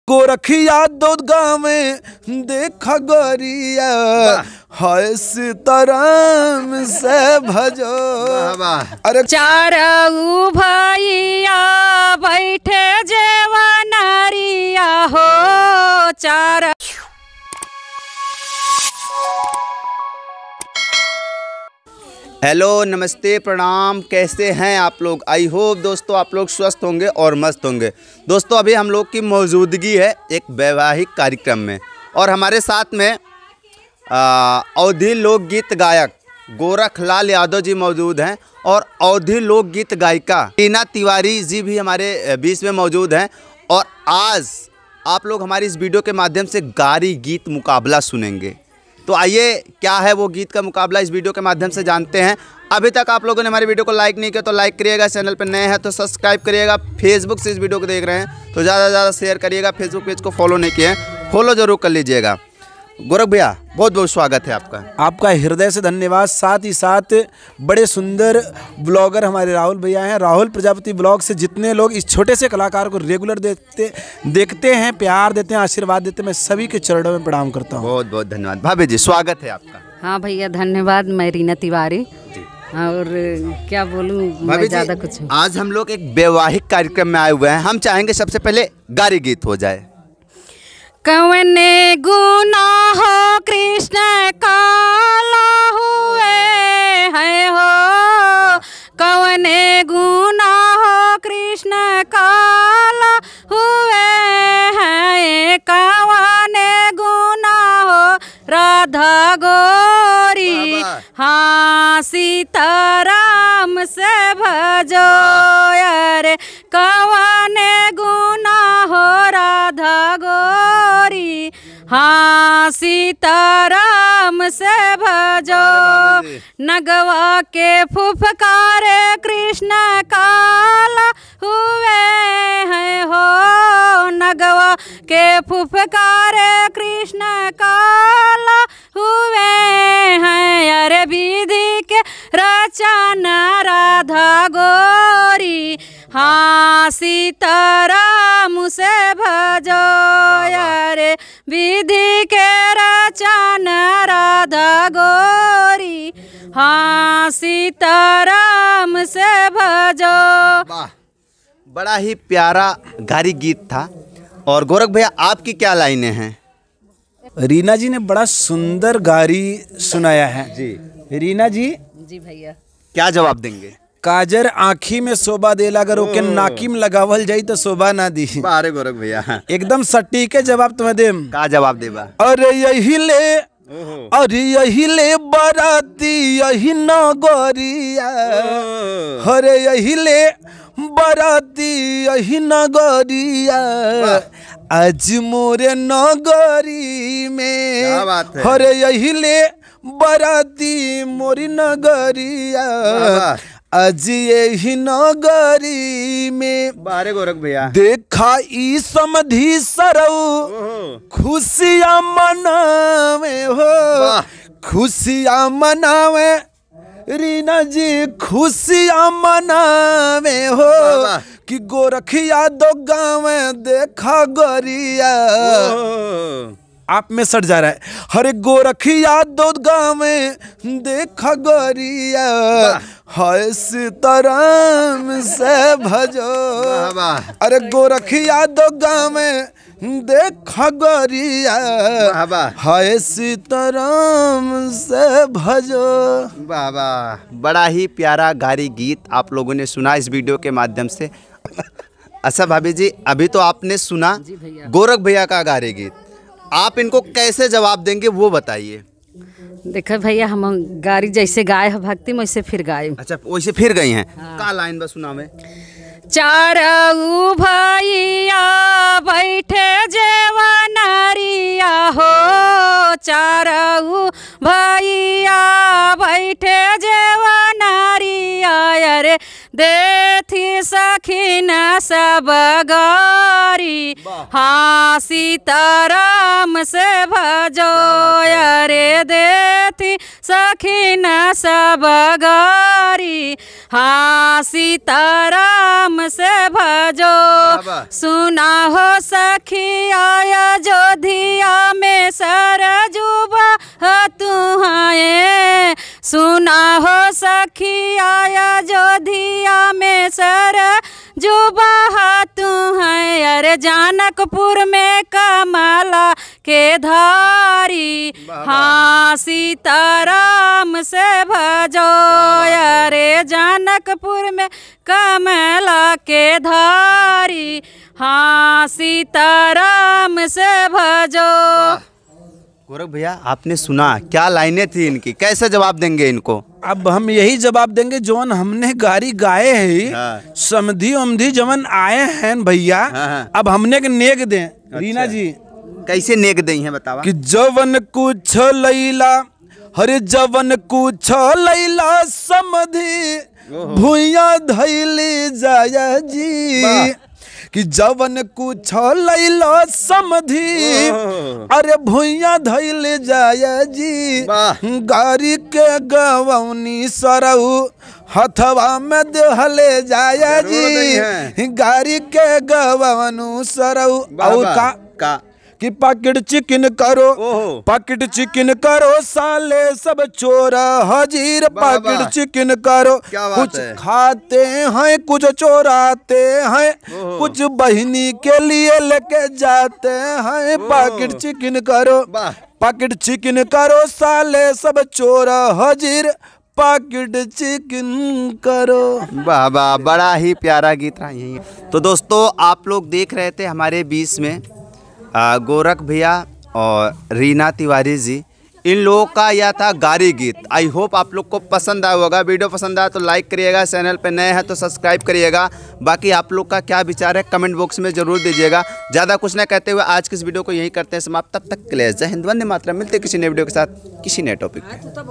Sadi video | गारी गीत मुक़ाबला-Gorakh Yadav, Reena twari | बंदना दूबे के यहा शादी समारोह | Rahul
Gari geet Muqabla
Gorakh Yadav ka video
Reena Tiwari ka video
Bandana Dube ki shaadi ka video
Banna Dube ke yahan shaadi mein क्या-क्या hua